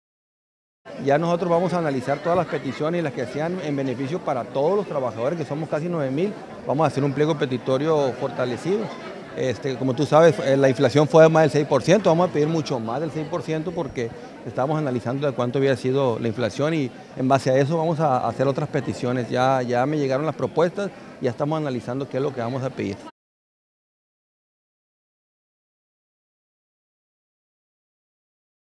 Prepara el STASE pliego petitorio
Dirigente del STASE  Gabriel Ballardo indicó que siguen preparando el pliego petitorio el cual darán los detalles  a finales de este mes.